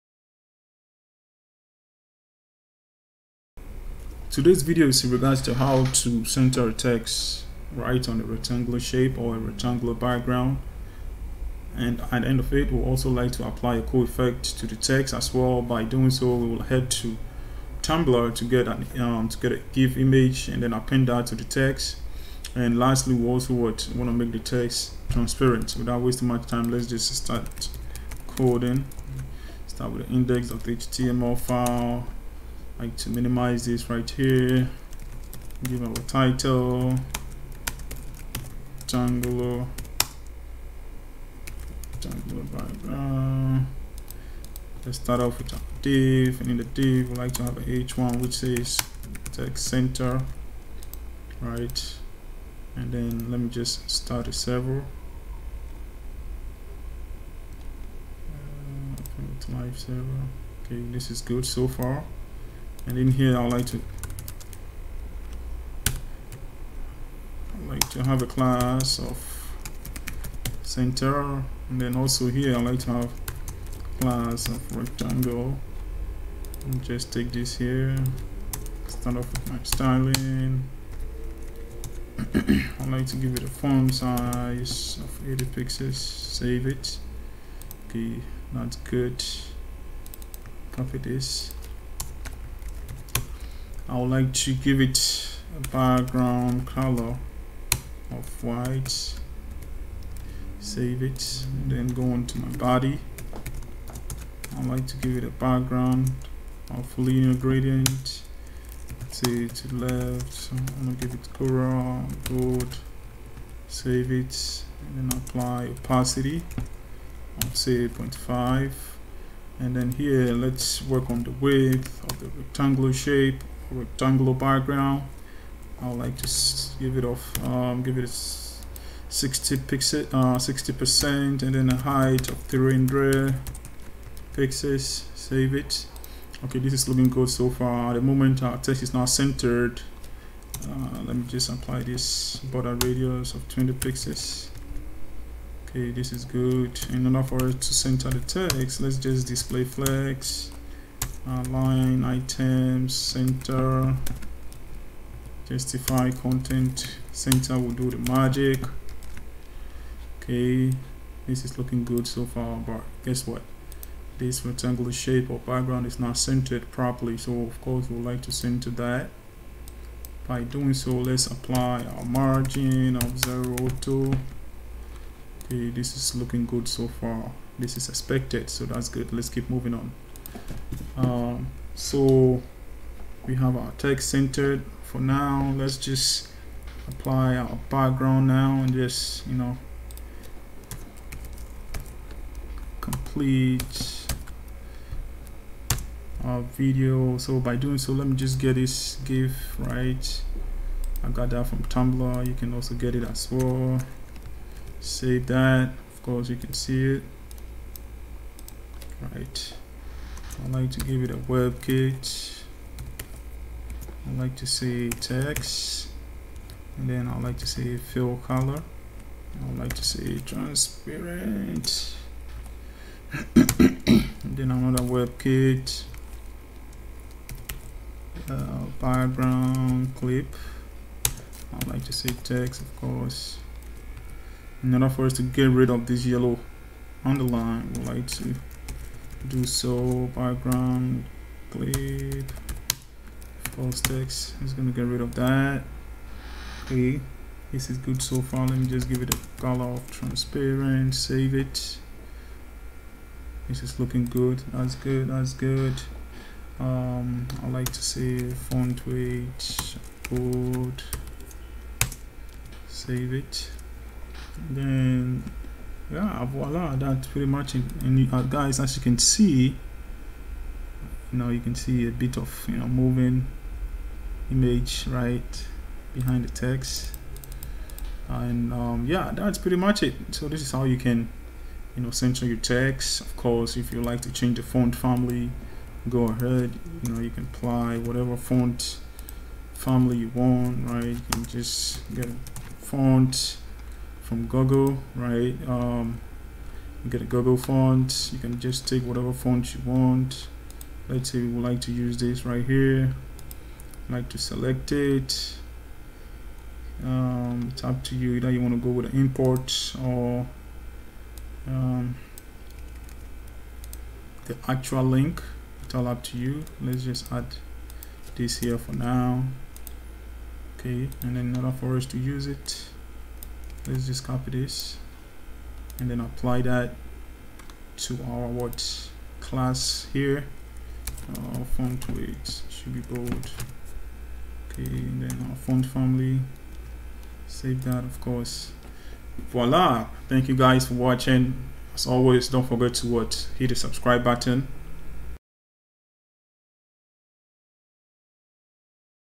Center a Text on a rectangular shape using Html, CSS
Video pertains to how to Center a Text on a rectangular shape or background using Html, CSS - Step-by-Step